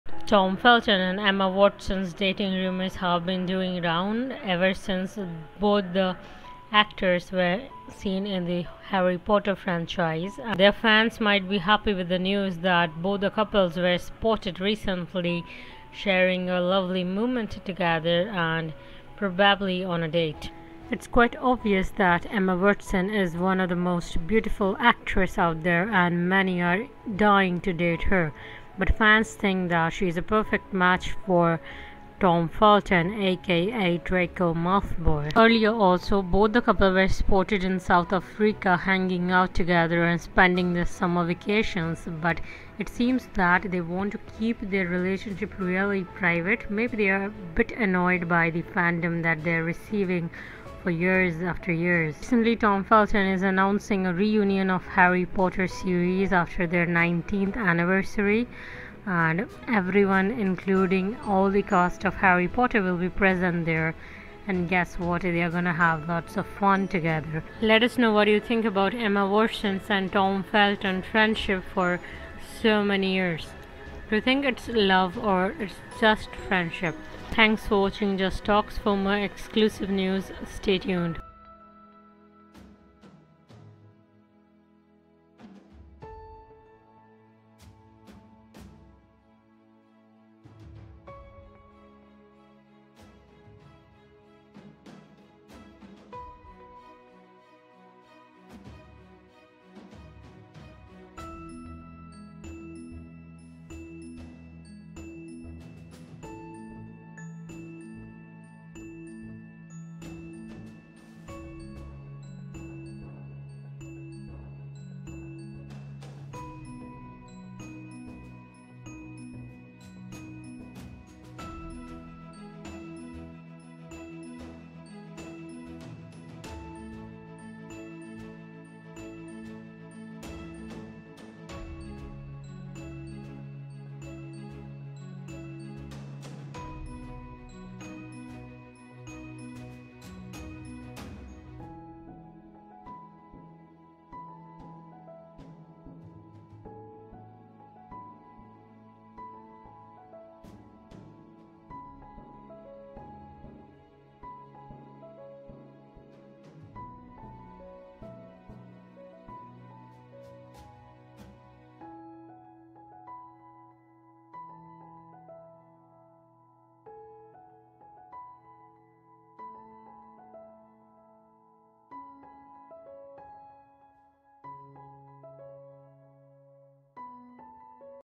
omg! Tom felton and Emma Watson spotted romancing on a date together
Voice Artist: Simmy